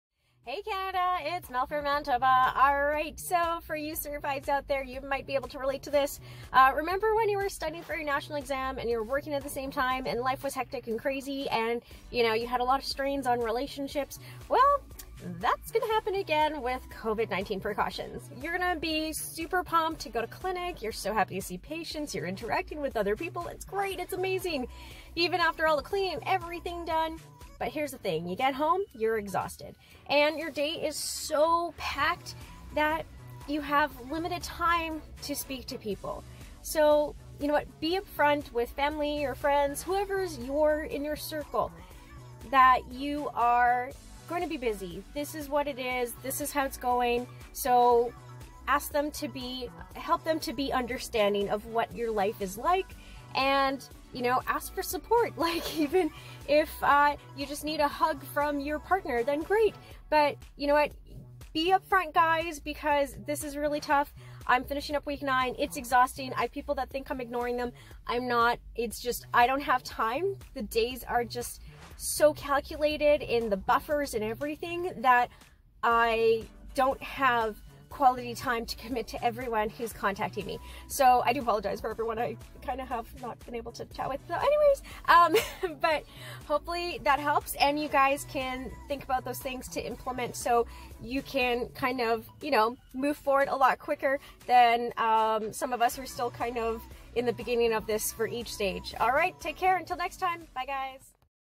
Medical Providers & Covid19
Lots of challenges happening with covid19 precautions. unfortunately many people aren't aware of the effects it's taking on them in their personal lives trying to keep up with constant daily changes.

Many of us are exhausted because we just can't keep up with the ever changing demands to practice as a medical provider.